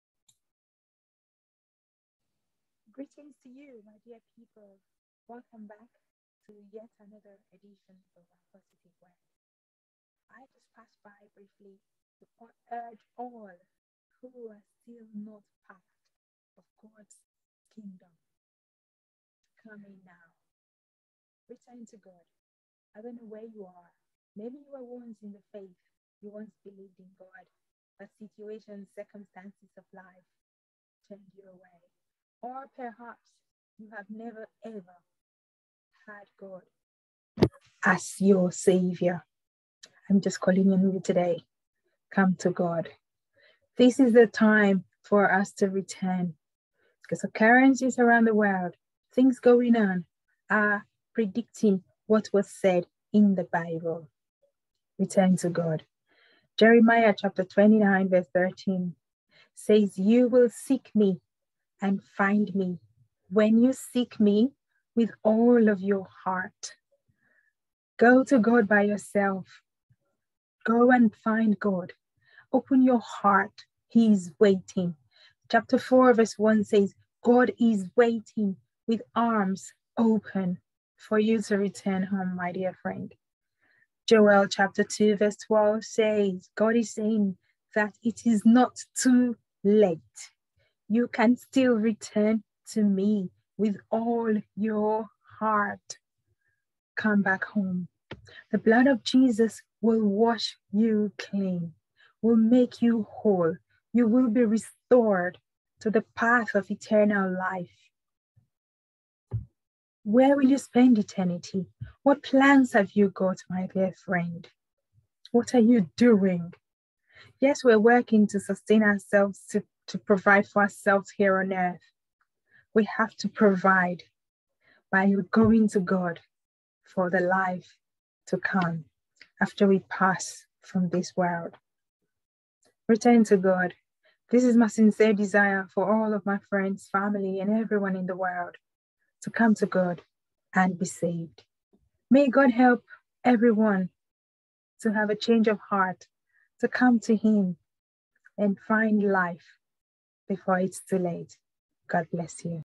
Day 336 Return to God
This is an invitation to all who may have known God in the past and went a different way or who may not have had a relationship with God at all. God says in Jeremiah 4:1 that He is waiting with open arms for you to come.

What will you give in exchange for your soul? Where do you intend to spend eternity? That plan can only be made through the decision you make while you are alive here on earth. Somebody created this world and all that are in it. One day, we will all be required to report to our Creator on how we spent the life He gave us. Will you be ready?

Delay no more. Jeremiah 29:13 You will seek Me and find me when you seek Me with all your heart. Joel 2:12 God says that it is not too late. You can still return to Me with all your heart. Where you are or what you may have done does not matter. You heavenly Father is expecting you. Will you keep Him waiting? Return to God. This opportunity to do so freely will not last forever. Do it while you can.

Apology for the sound at the beginning. That is still a work in progress. I really appreciate your understanding. Thanks.